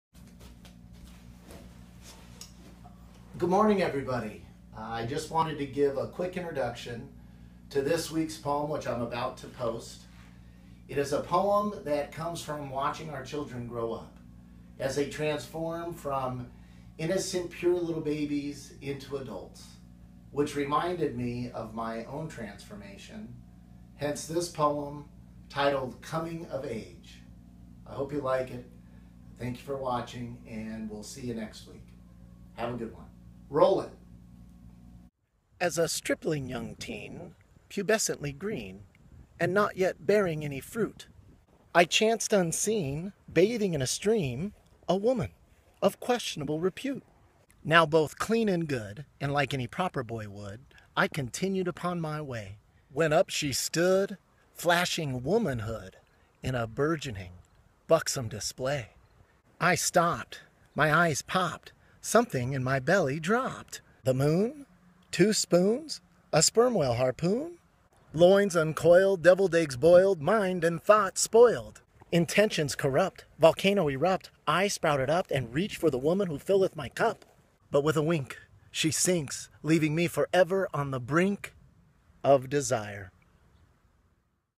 "Coming of Age" by the Fibonacci Monkey
Original poem "Coming of Age" by Michael Collins the voice of the fibonacci monkey poems.